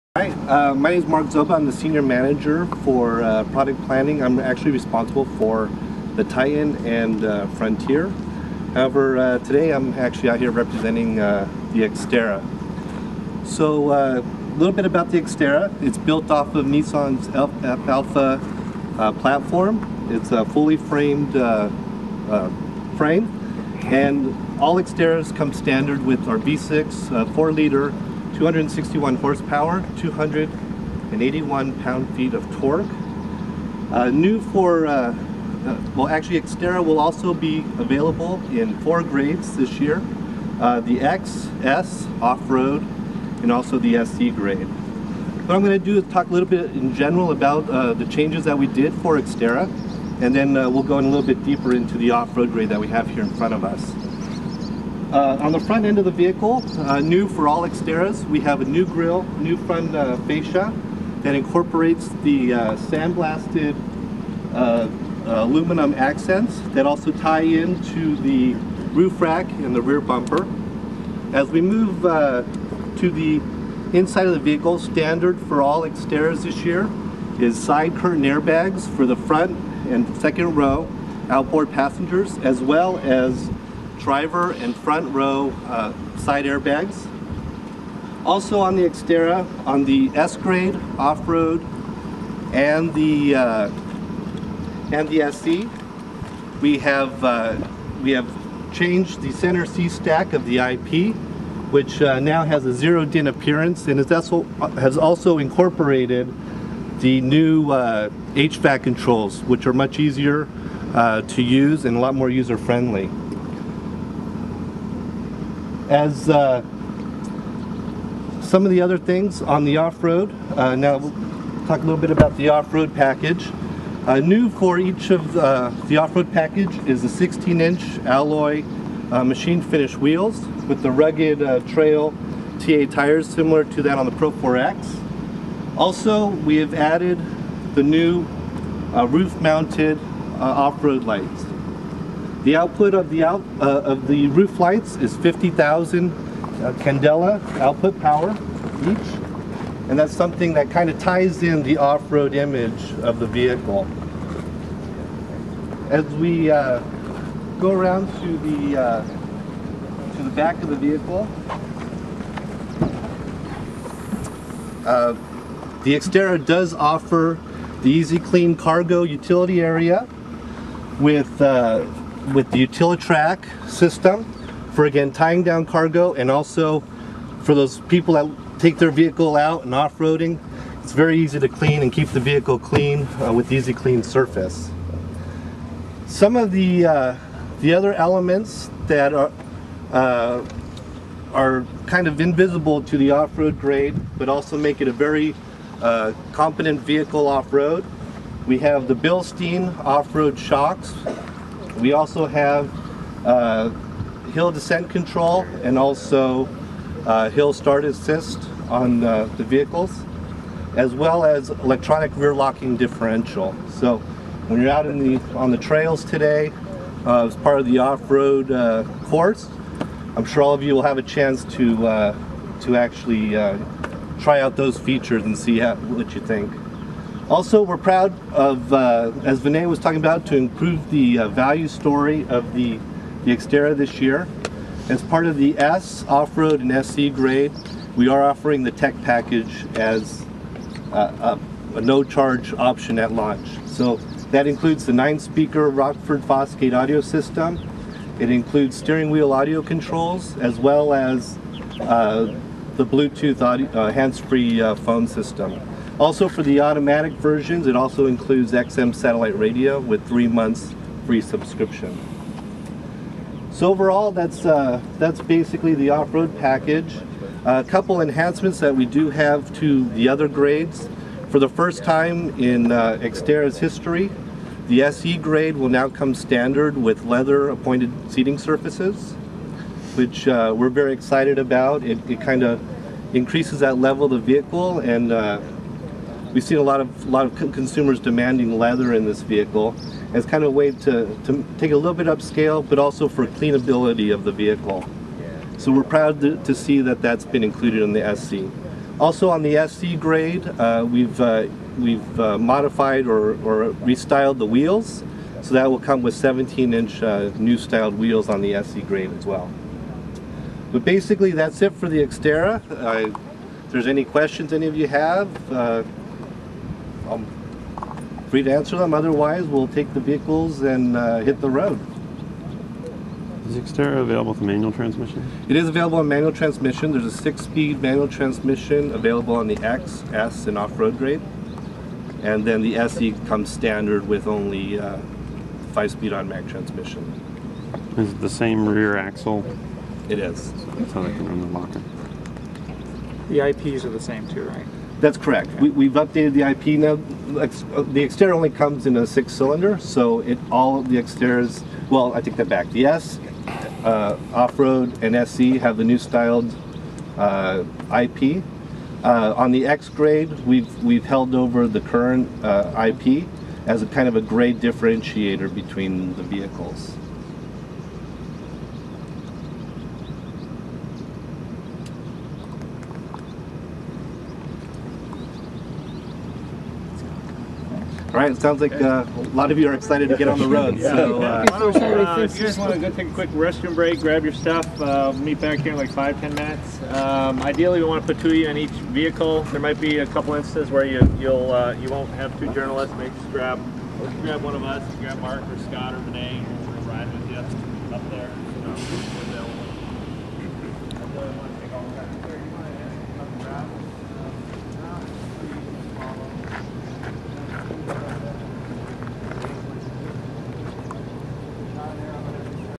Xterra Walk Around, Nissan trucks 2009.
Walk around of Xterra at media introduction for 2009 Nissan Trucks & SUVs held in Columbia River Gorge, WA. Aug. 25-26, 2008.